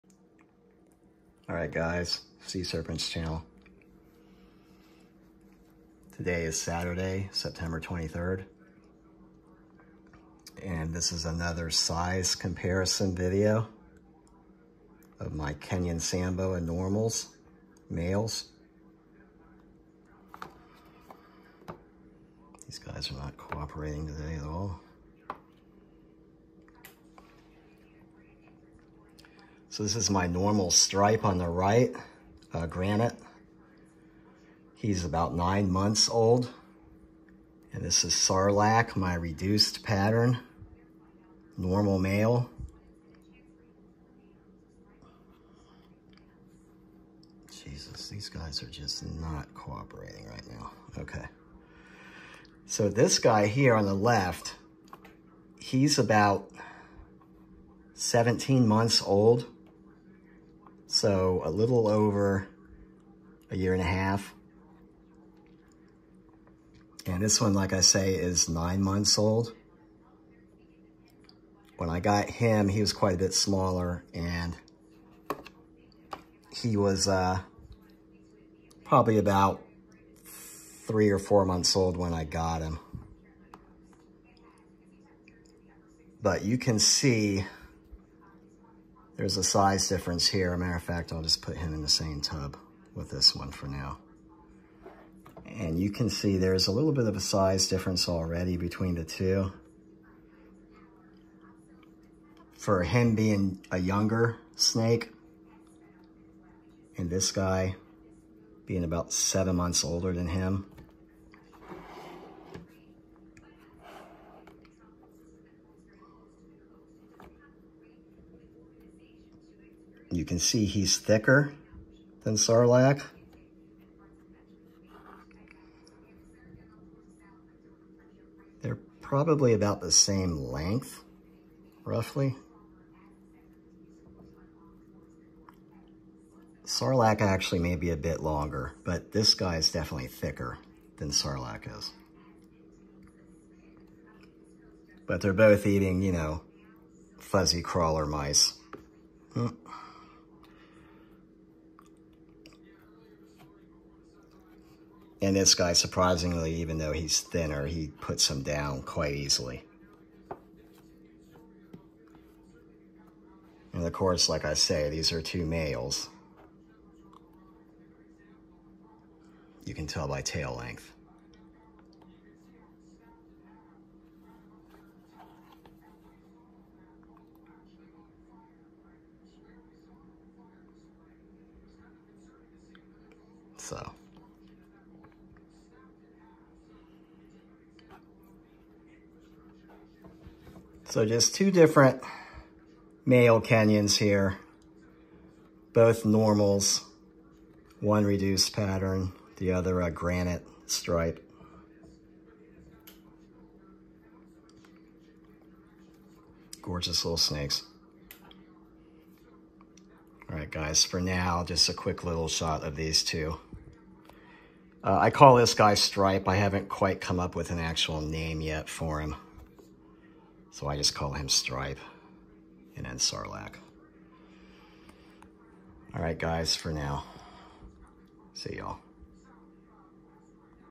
Kenyan sand boa normal males:reduced pattern and granite stripe.
Quick look at my two smaller sand boas.